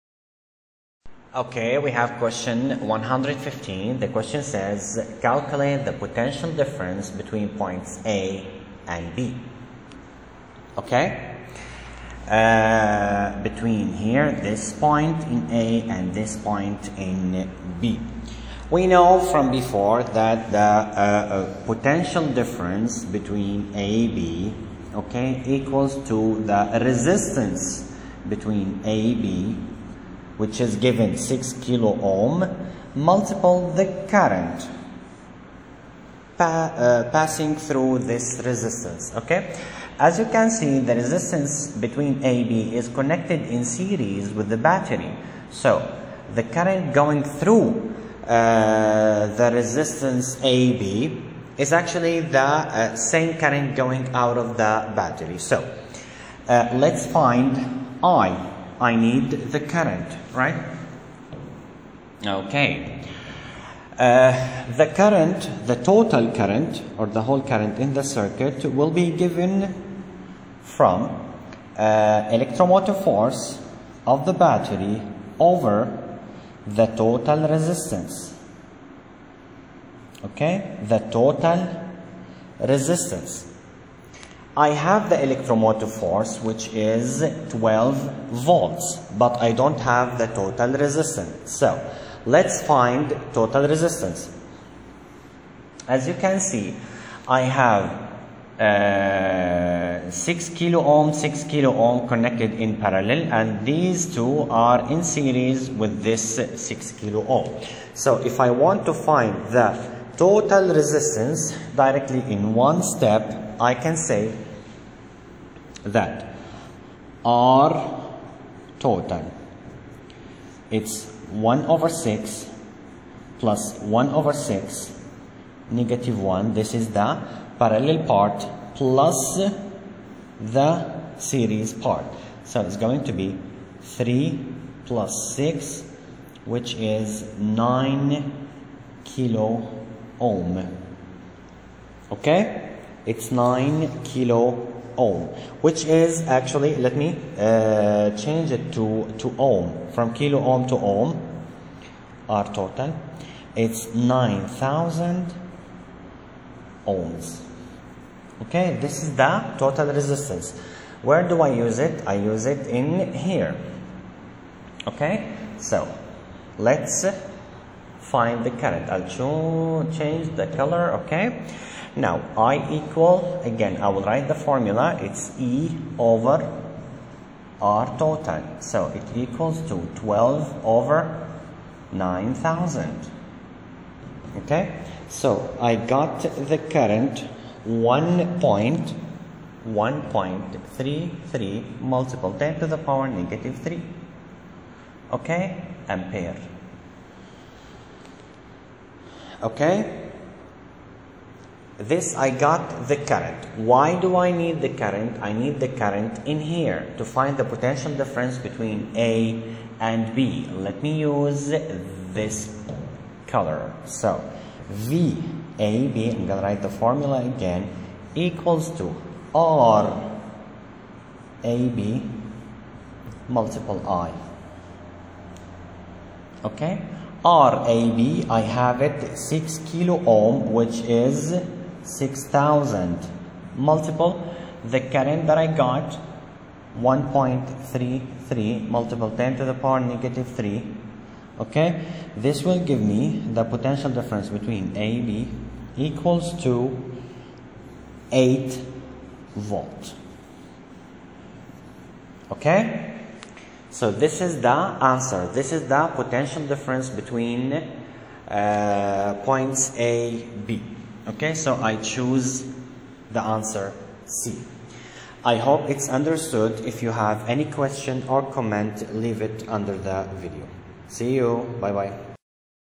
115) Calculate the potential difference between points A and B is
115) Calculate the potential difference between points A and B is
A) 4v                                          B) 6v
C) 8v                                          D) 12v